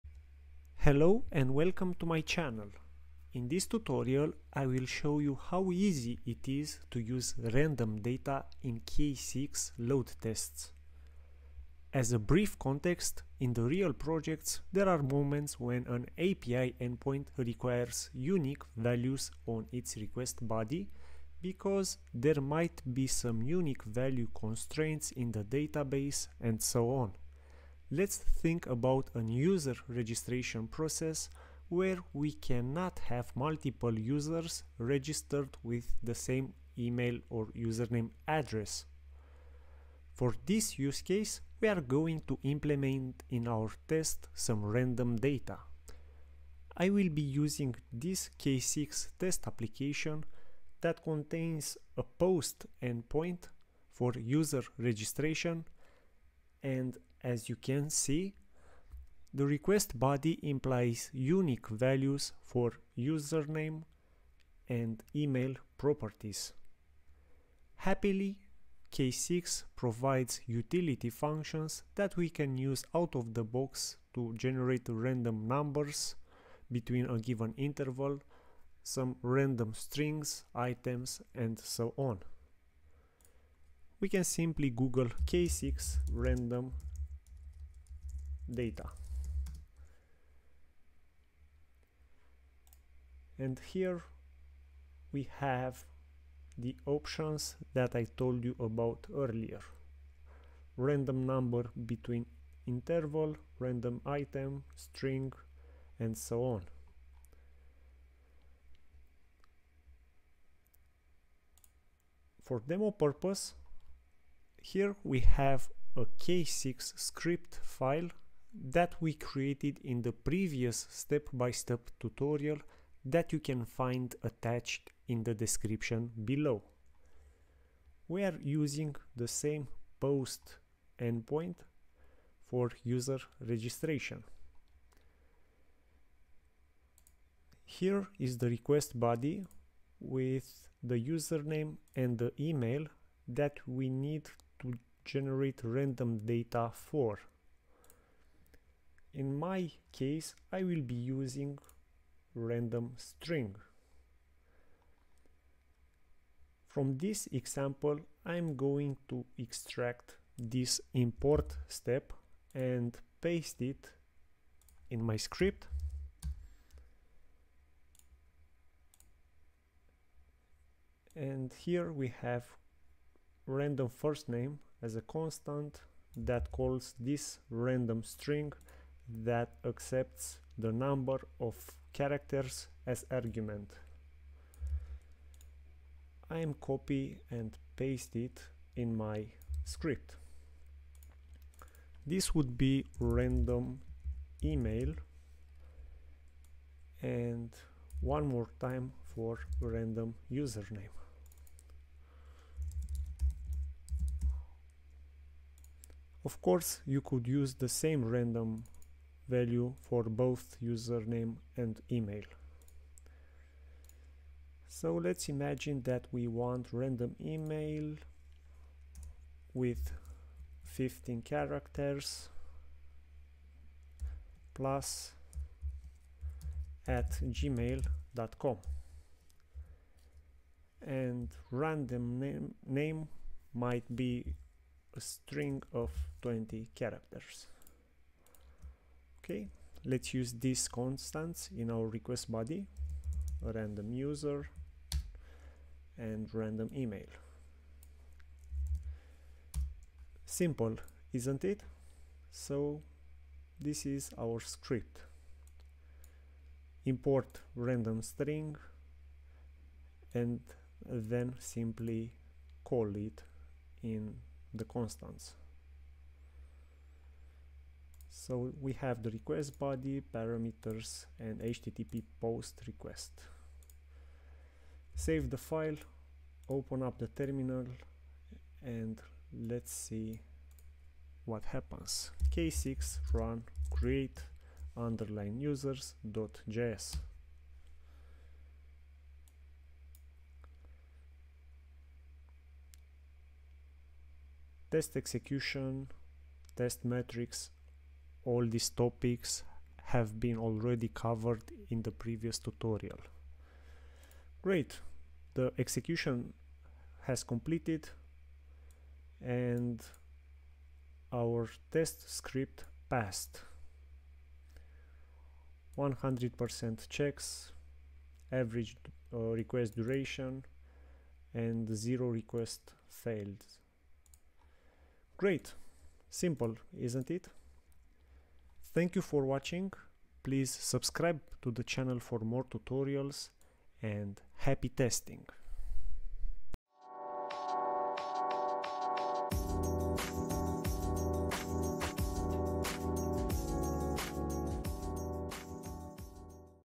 k6 Tutorial - how to generate Random Data in load tests
In this quick video we will learn how to generate random data in k6 load test scripts